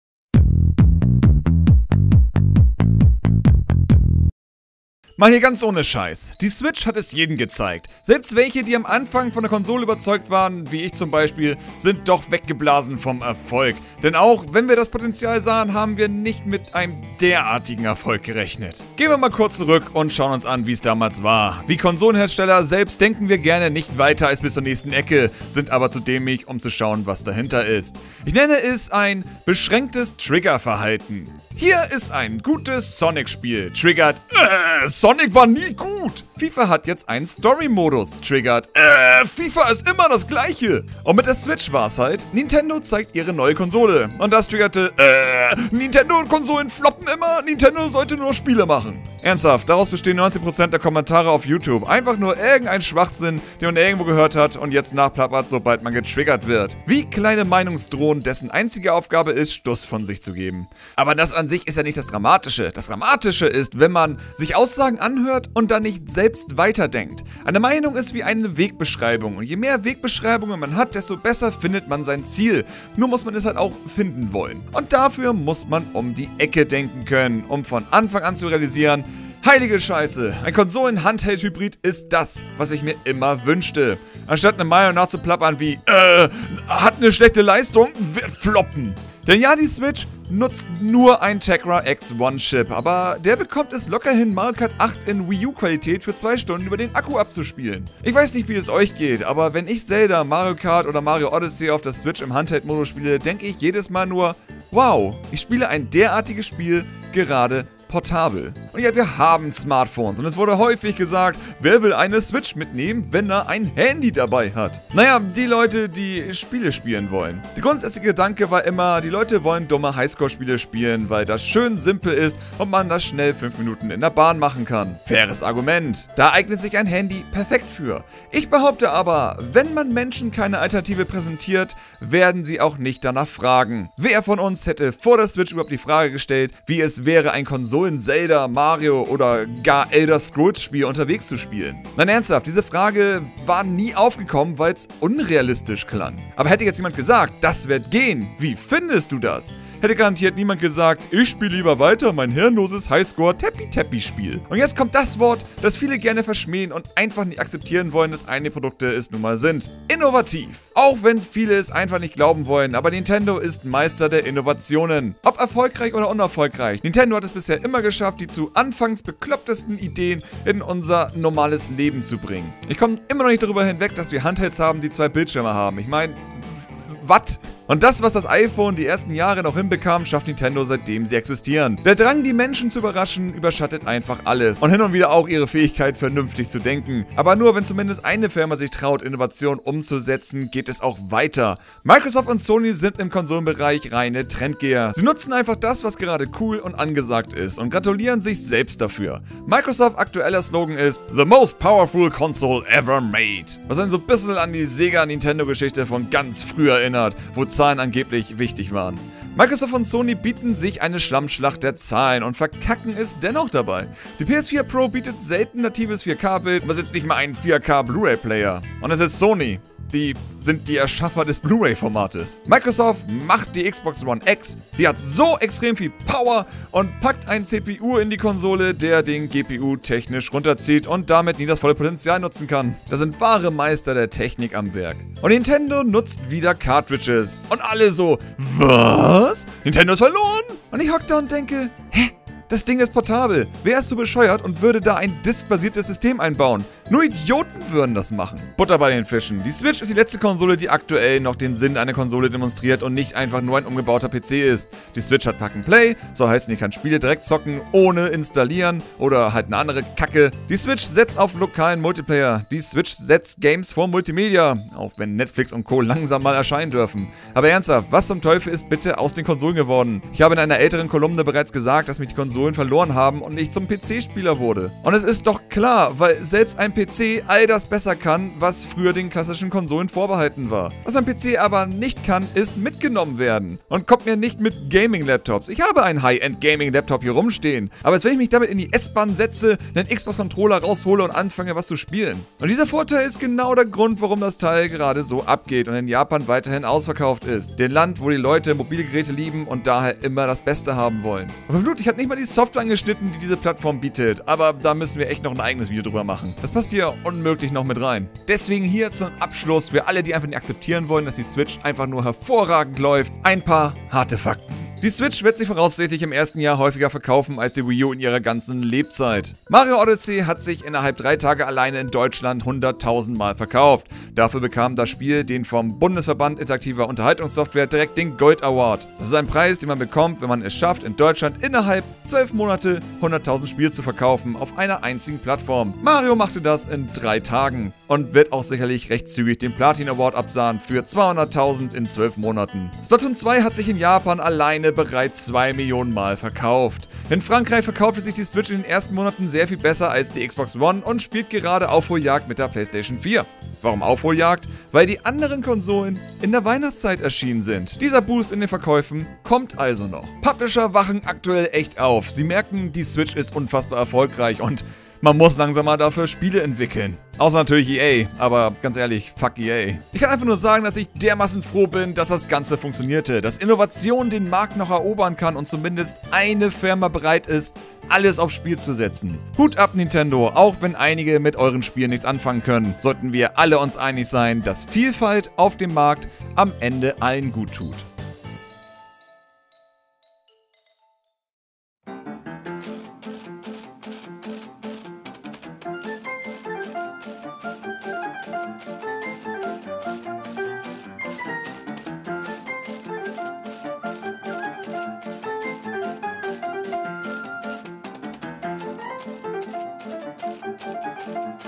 Die Nintendo SWITCH ZERSTÖRT die Konkurrenz | Kolumne
Viele wollten es nicht glauben, einige glaubten nicht in dem Rahmen, aber die Nintendo Switch ist ein Wahnsinns Erfolg!

🔊 Werde GamingClerks VIP - bekomme coole Extras & supporte uns so direkt! Klicke einfach hier drauf und dann auf den Button "VIP werden"